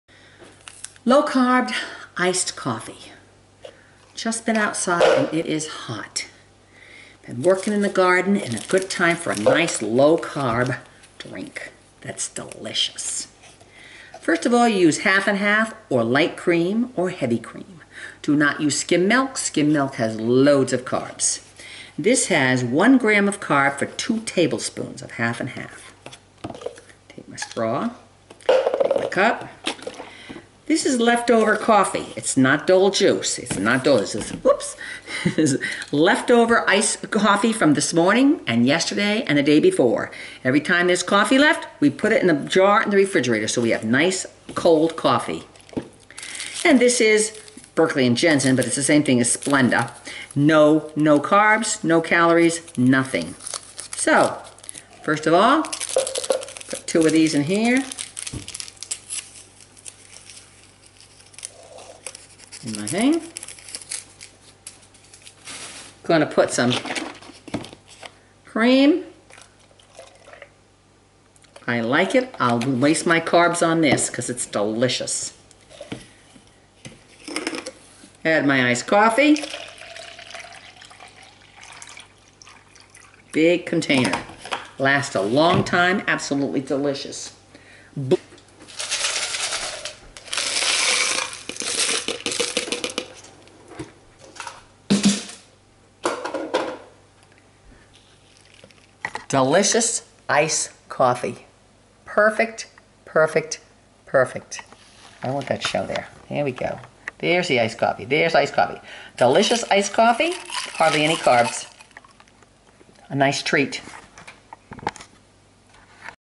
Low Carb Iced Coffee
Cold leftover coffee
Splenda
half and half
crushed ice